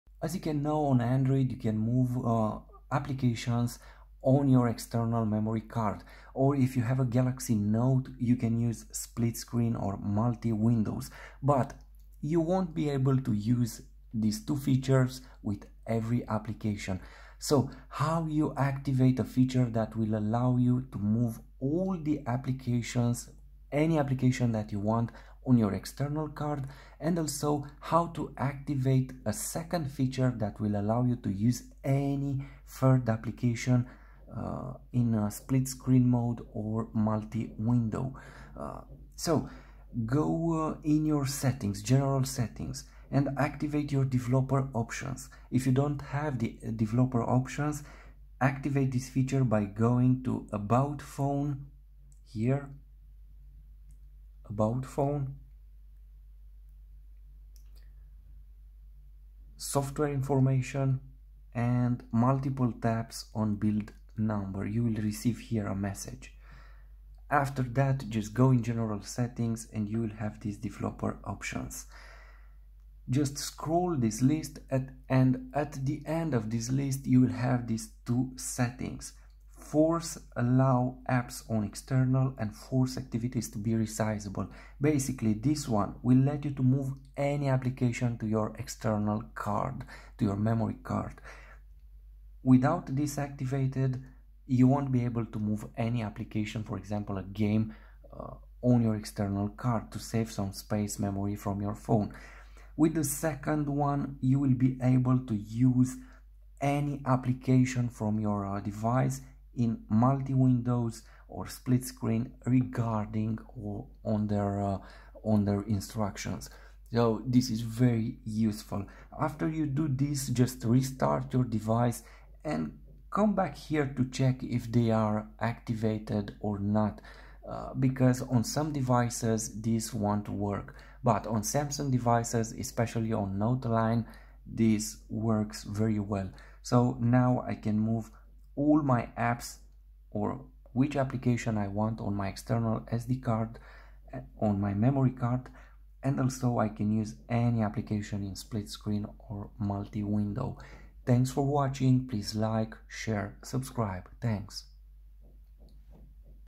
Force allow all apps on external card & force all apps to be resizable on NOTE 10+
Force allow all apps on external card & force all apps to be resizable